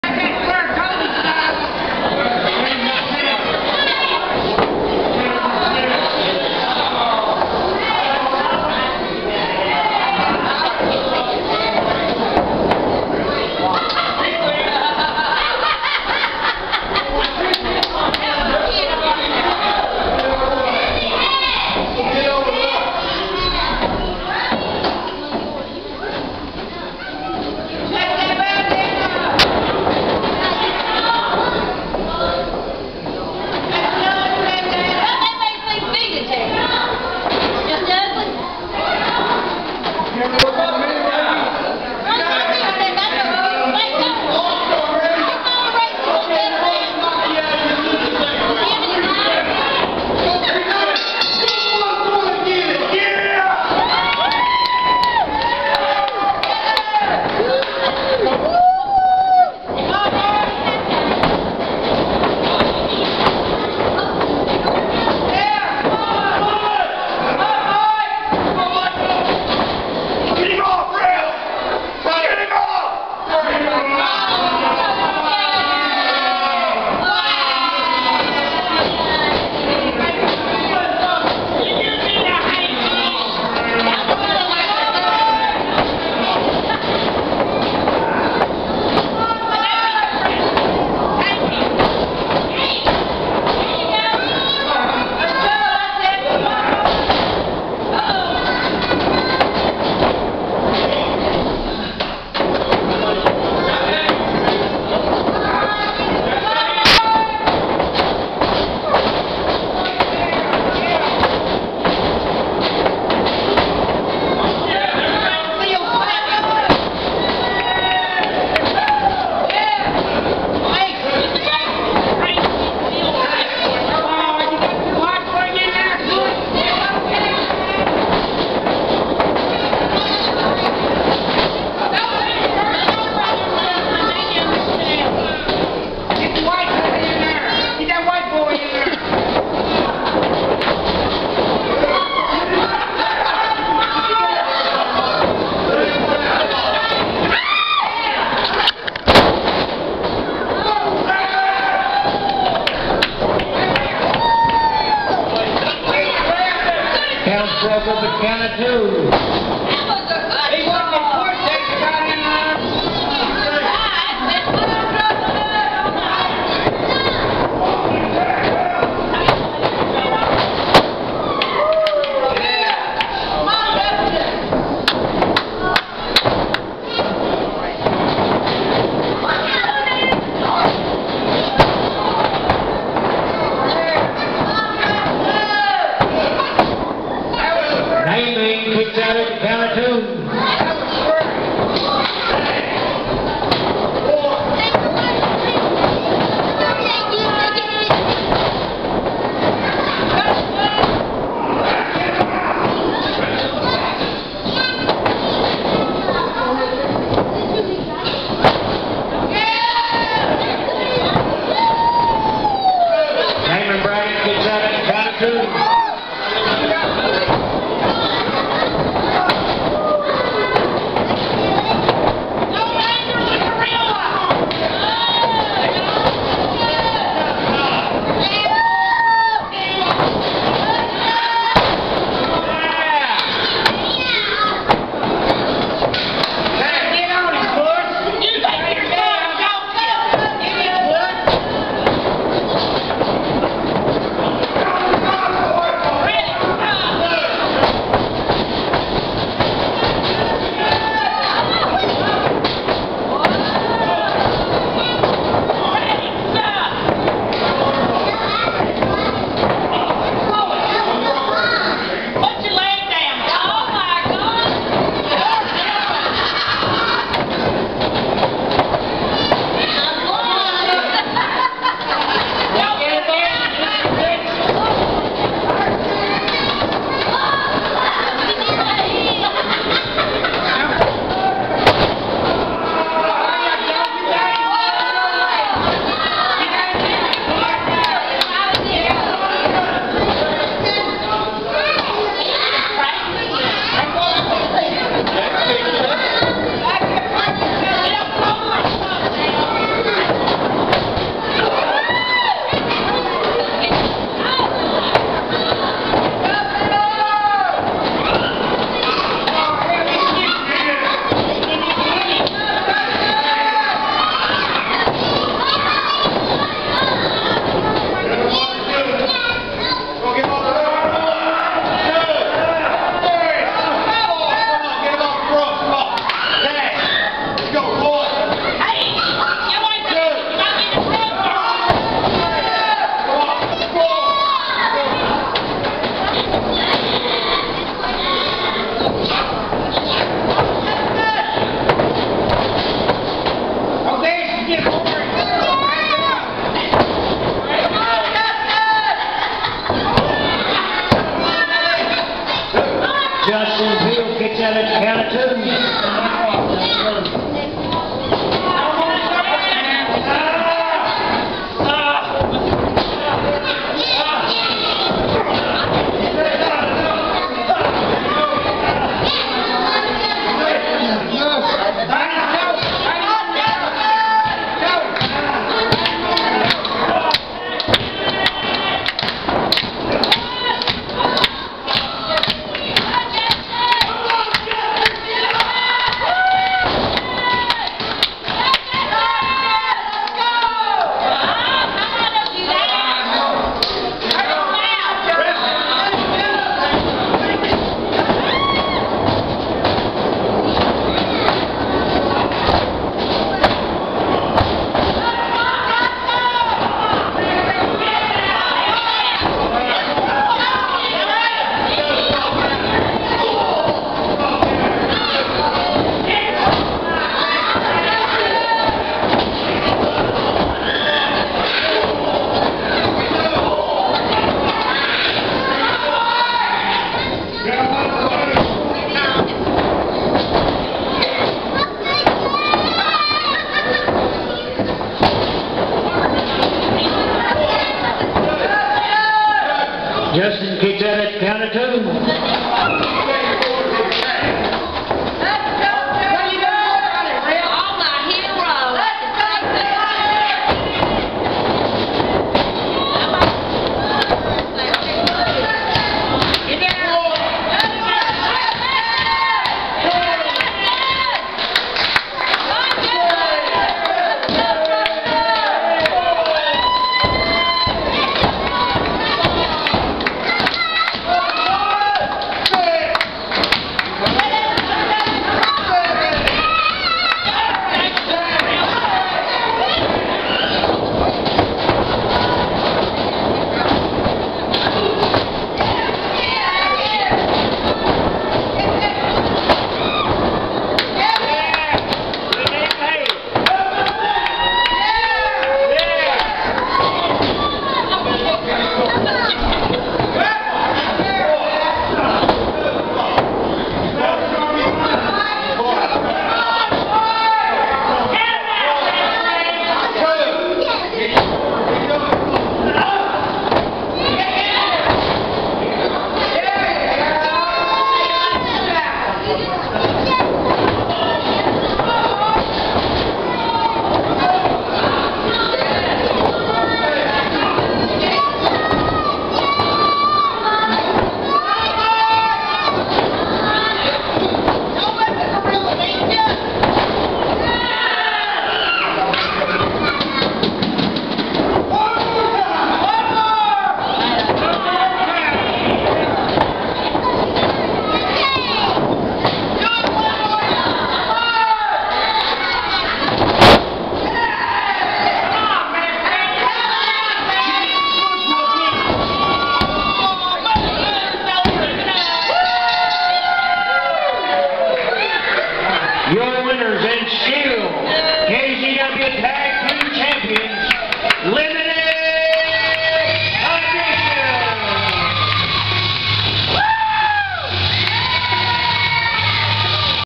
Limited Edition vs Sully Larkins and Damon Braddock @ KZW 5-5-2012
May 5, 2012 Limited Edition once again defending their tag titles.  KZW @ Liberty, Ky 5-5-2012